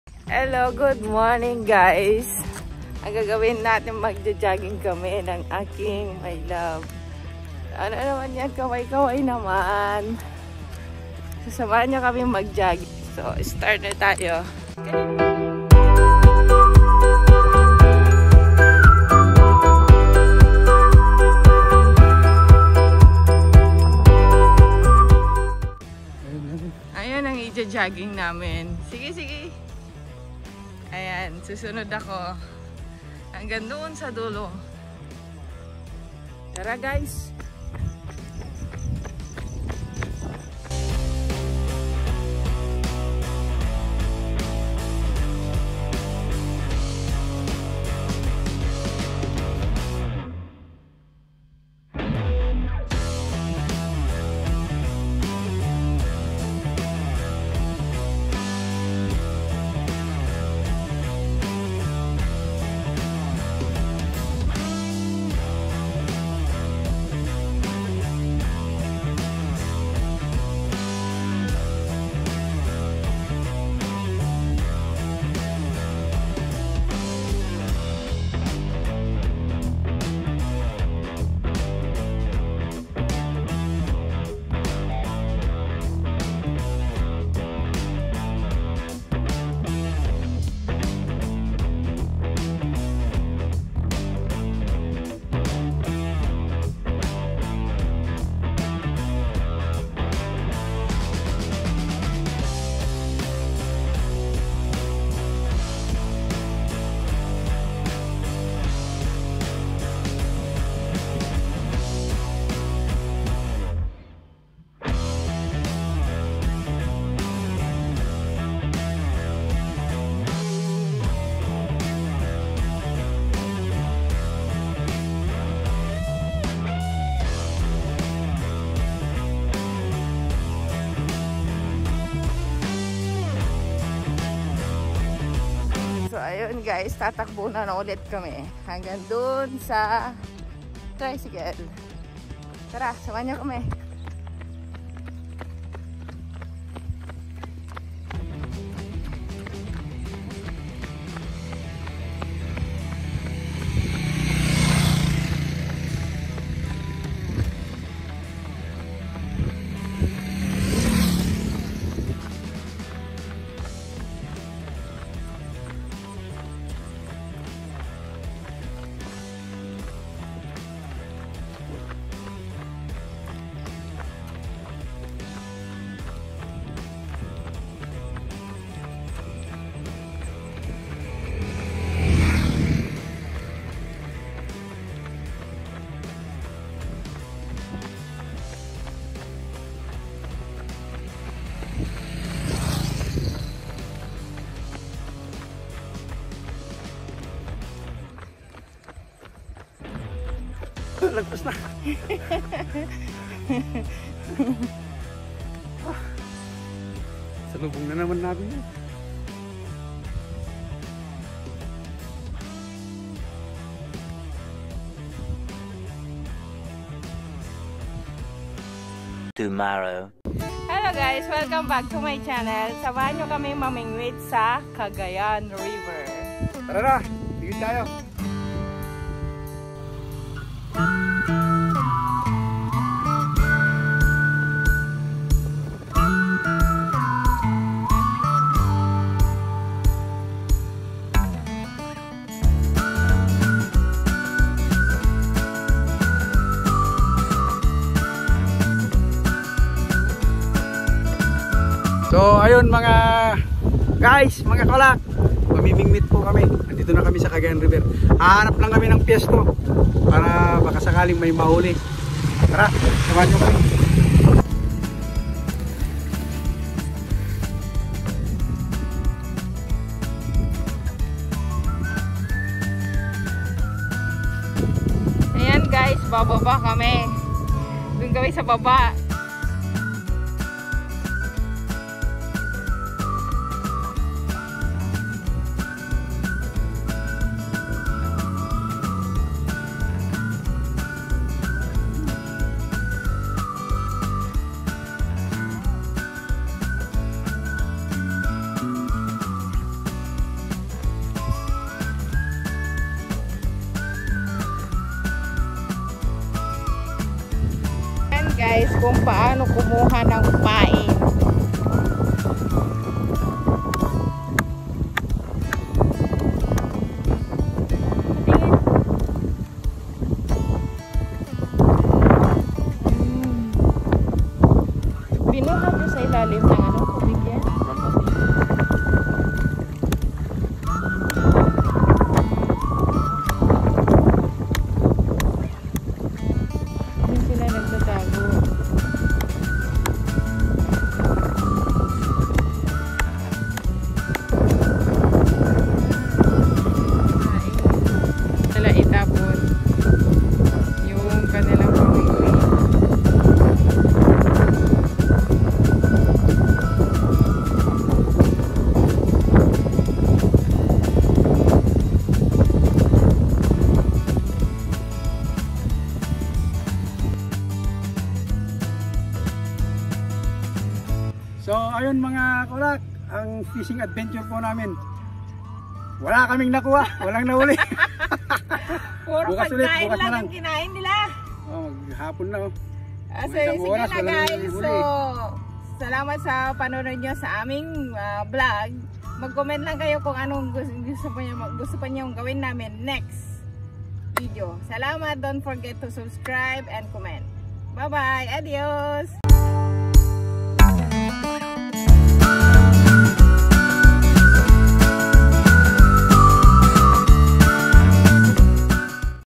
BUHAY PROBINSYA | by: NANCY N. ACOSTA
Hello guy's simple lang buhay sa probinsya, nag-jogging kami ng asawa ko at sa hapon naman namingwit kami sa Cagayan river.